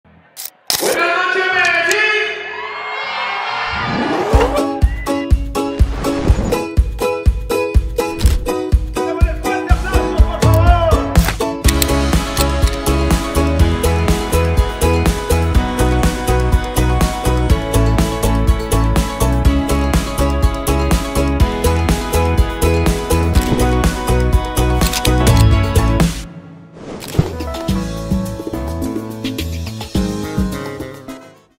Escenario La 70 Norte [Feria en TM] Telemedellín
En el Escenario La 70 Norte bailamos, cantamos y disfrutamos la FeriaDeLasFlores2023 de principio a fin 💃🕺.